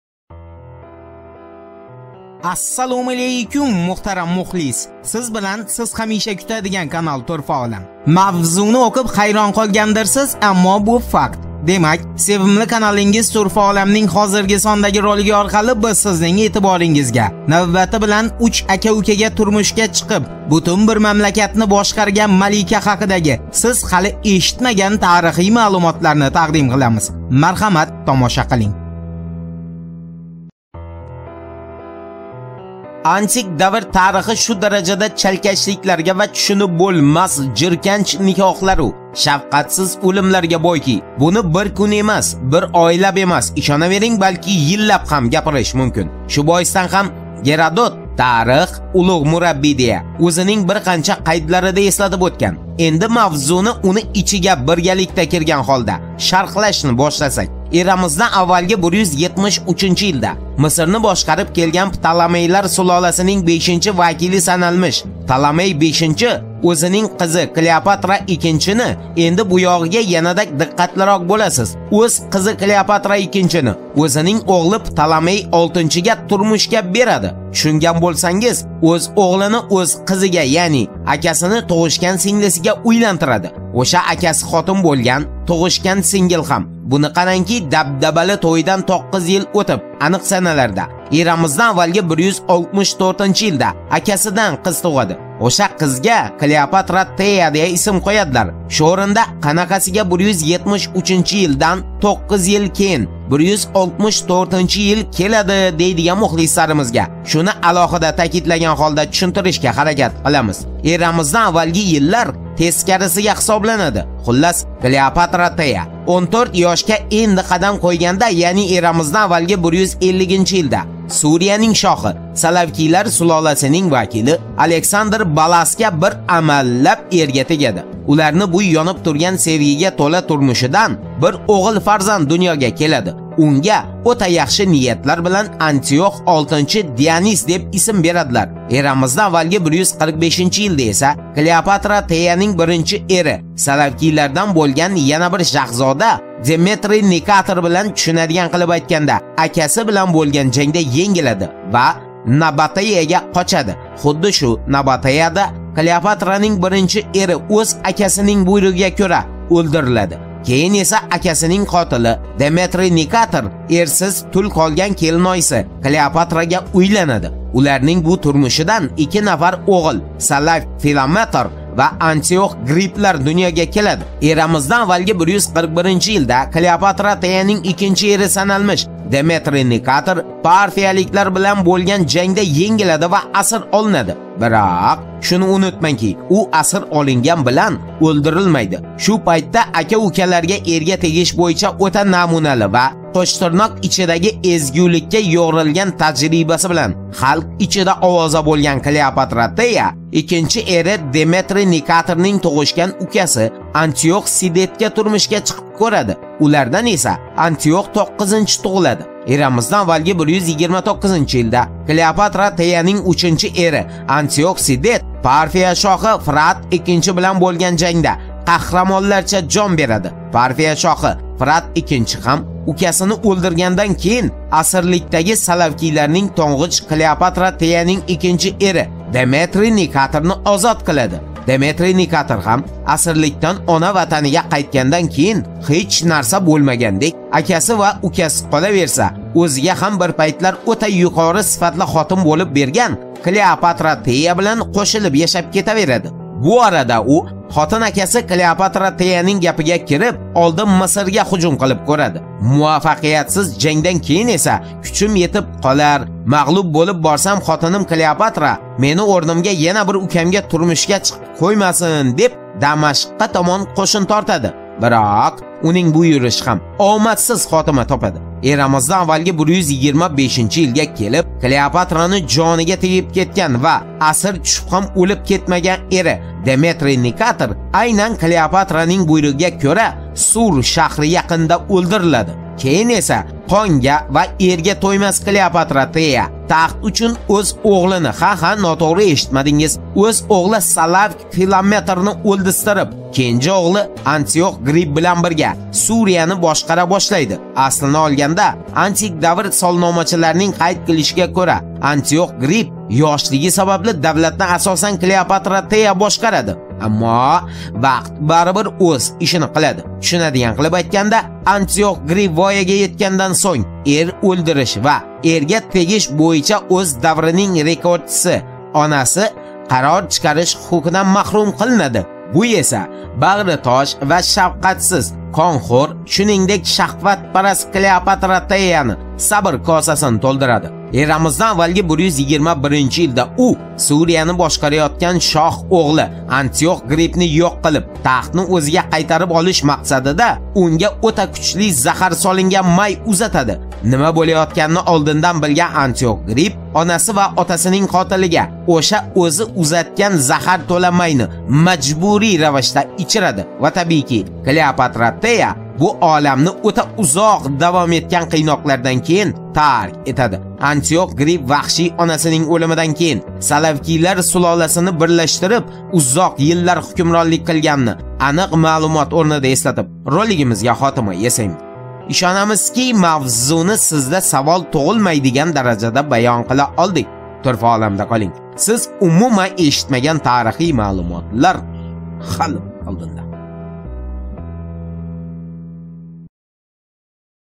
АКА УКАЛАРГА ЭРГА ТЕГИШ БУЙИЧА РЕКОРДЧИ МАЛИКА
Ассалому алайкум мухтарам Турфа Оламнинг кадрли ва азиз мухлислари.Ушбу сондаги ролигимиз оркали биз сизнинг эътиборингизга навбати билан уч ака укага турмушга чикиб бутун бир мамлакатни бошкарган малика хакидаги сиз хали эшитмаган тарихий маълумотларни такдим киламиз.Мархамат тамоша килинг.